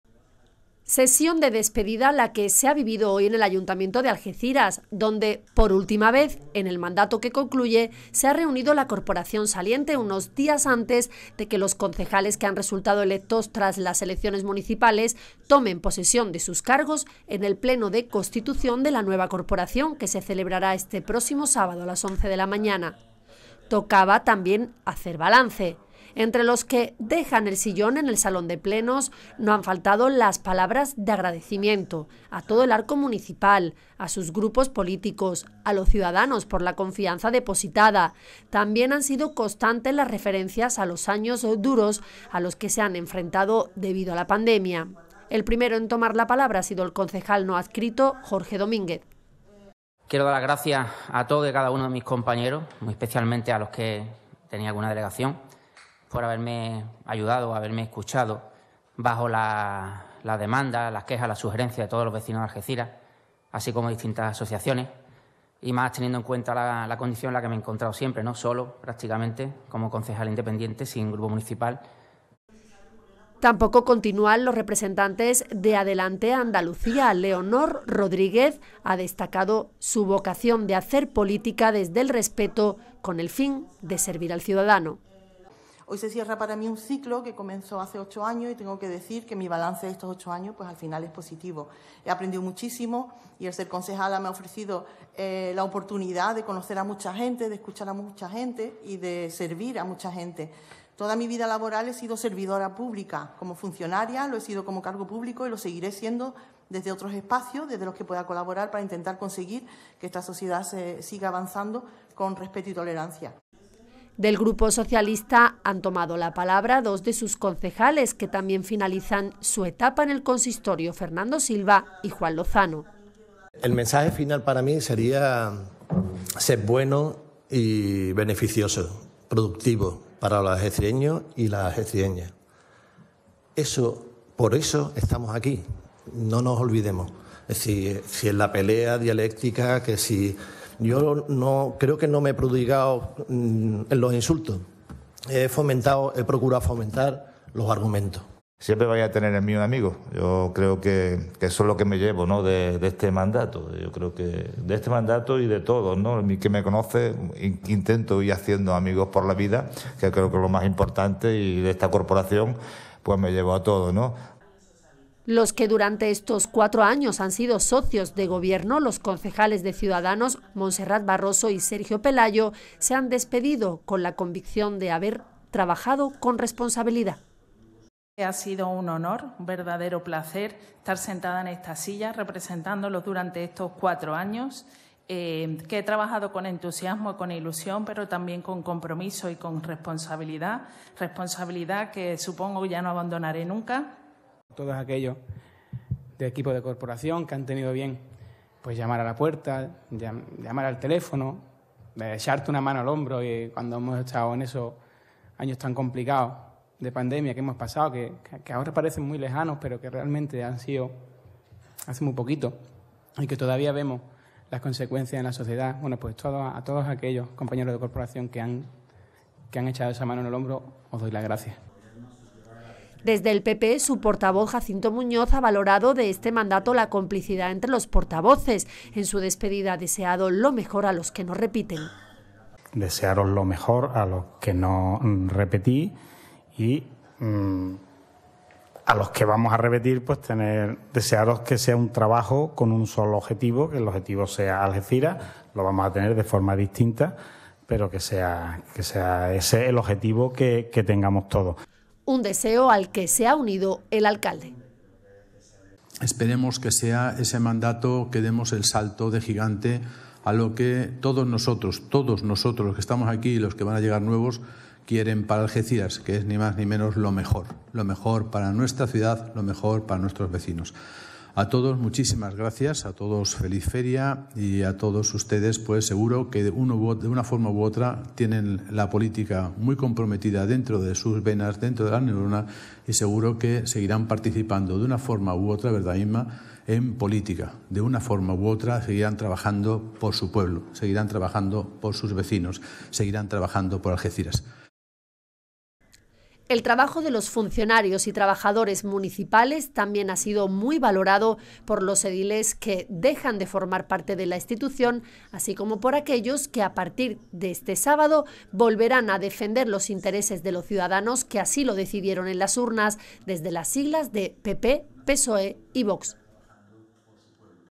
La corporación celebra el último pleno con la despedida de los concejales que no continúan
La corporación en funciones se ha reunido hoy para celebrar el último pleno del mandato antes de de la constitución de la nueva este sábado. Una sesión de despedida para los concejales que no continuarán durante los próximos cuatro años y de buenos deseos para quienes seguirán ocupando un sillón en el salón de plenos.